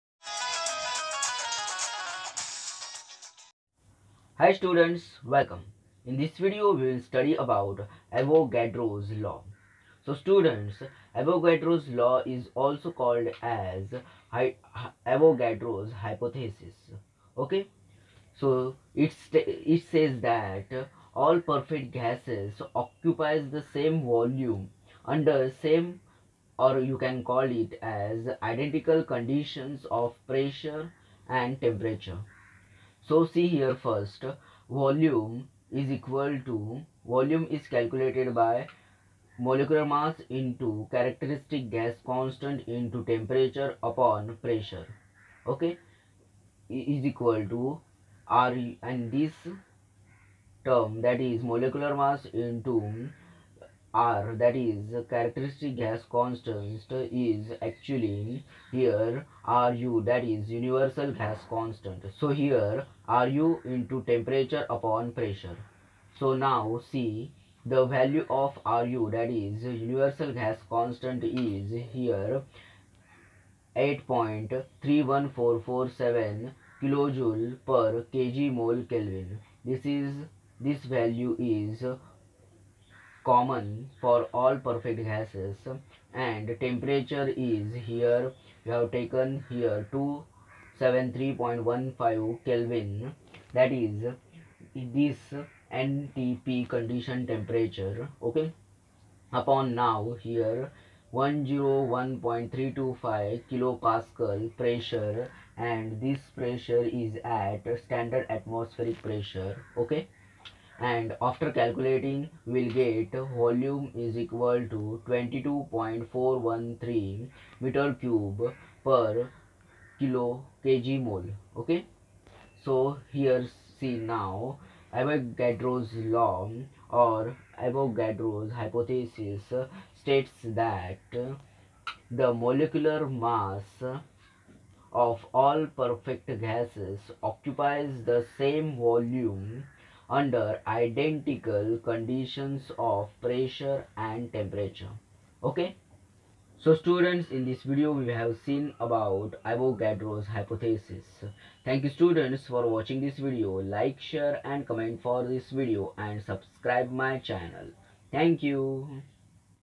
Avogadro's Law | Avogadro's Hypothesis | Thermodynamics
Hi Students... Welcome !!!
I am Pratik Pagade.

1) What is Avogadro's Law ?
2) What is Avogadro's Hypothesis ?

Pratik Pagade
PSP